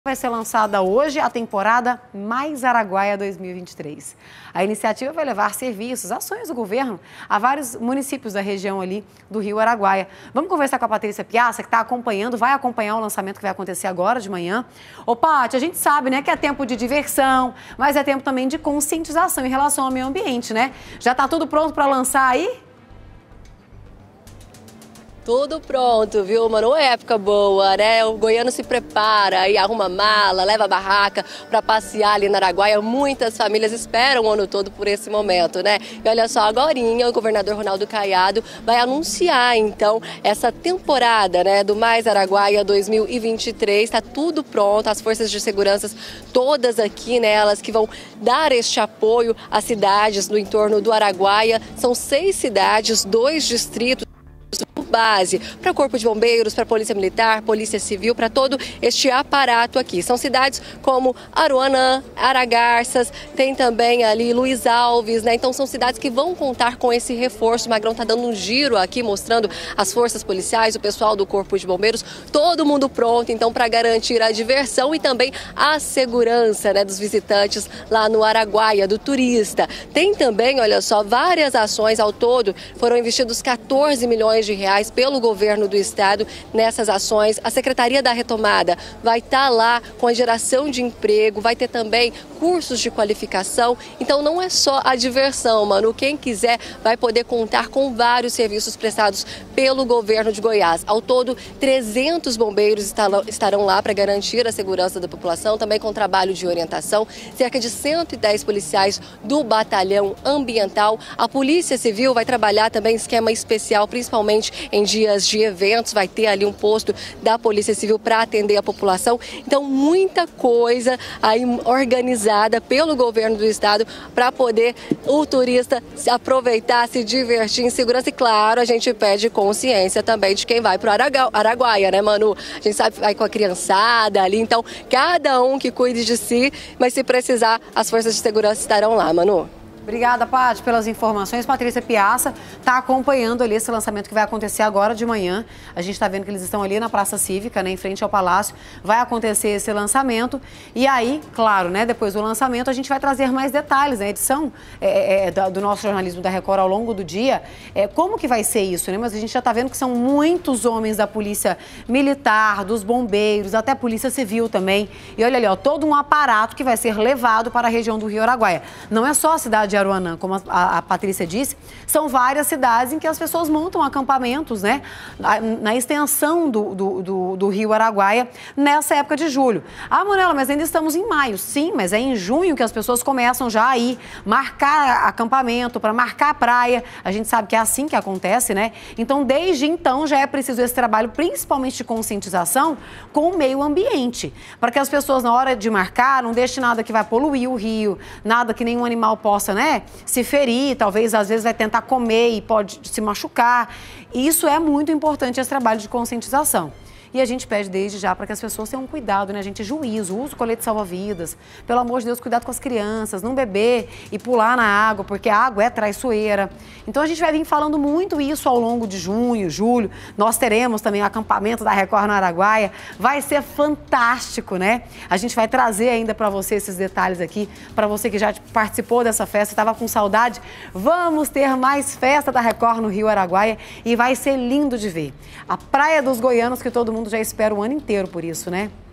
ARAGUAIA 2023: ESTÁ ABERTA A TEMPORADA DA PRAIA DOS GOIANOS
VAI SER LANÇADA HOJE A TEMPORADA MAIS ARAGUAIA 2023. A INICIATIVA VAI LEVAR SERVIÇOS E AÇÕES DO GOVERNO DE GOIÁS A OITO MUNICÍPIOS NA REGIÃO. PATRICIA, É TEMPO DE DIVERSÃO, MAS É PRECISO CUIDADO COM O MEIO AMBIENTE.